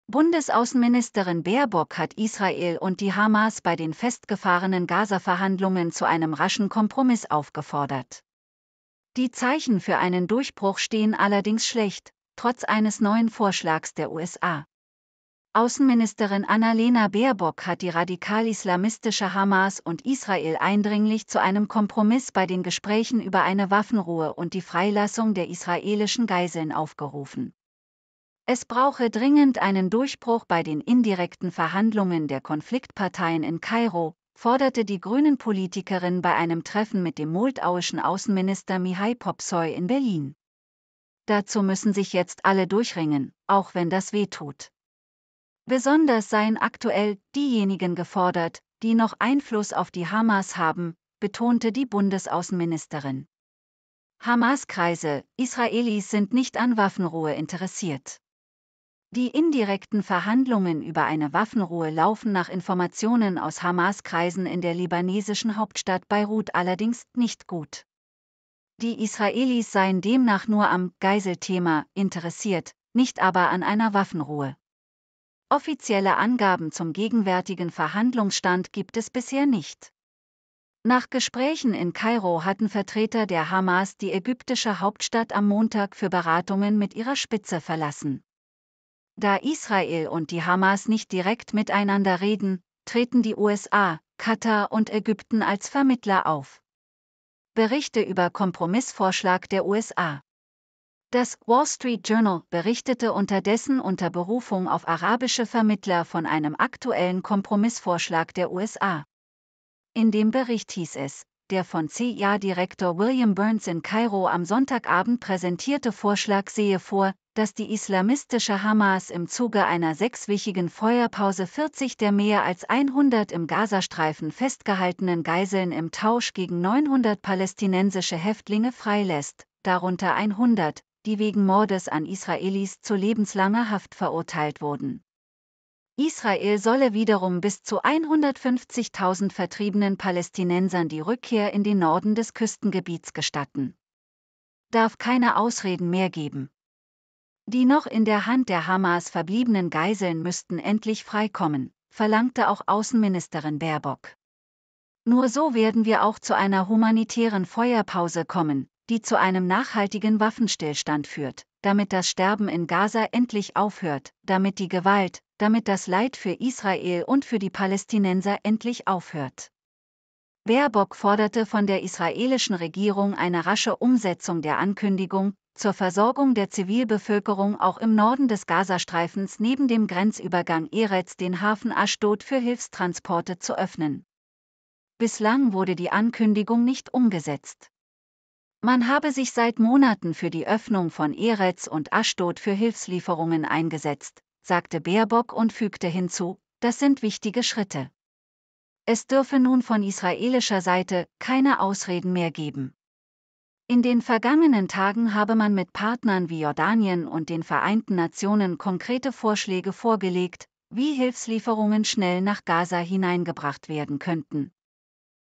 Gaza-Verhandlungen: Eindringliche Forderungen, kaum gute Zeichen
Bundesaußenministerin Baerbock hat Israel und die Hamas bei den festgefahrenen Gaza-Verhandlungen zu einem raschen Kompromiss aufgefordert.